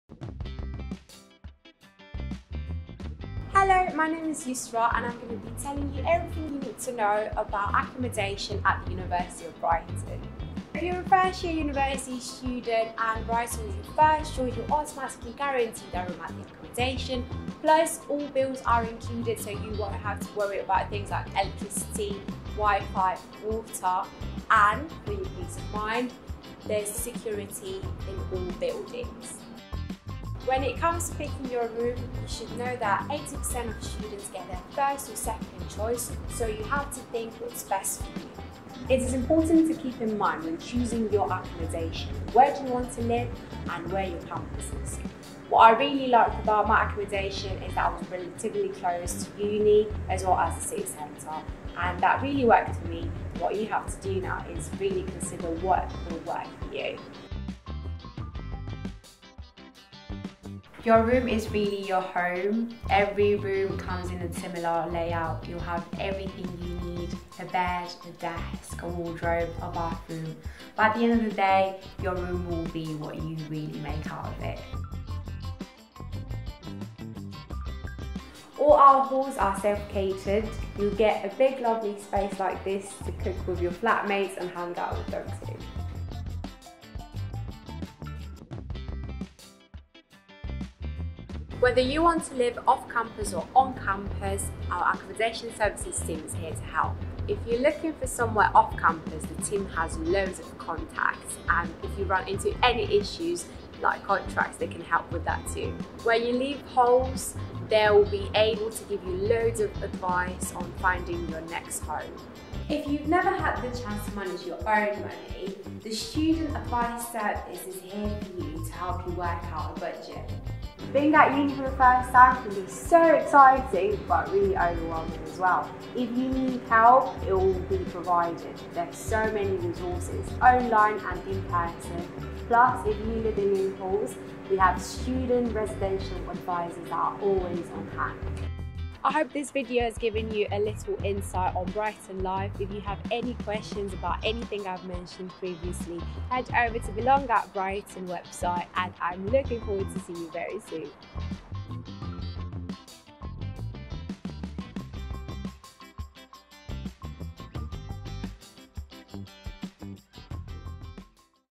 University of Brighton Halls of Residence: Some Need-To-Knows
The University of Brighton’s halls of residence are a key aspect of student life at Brighton – learn about all the things you need to know by hearing from one of our students.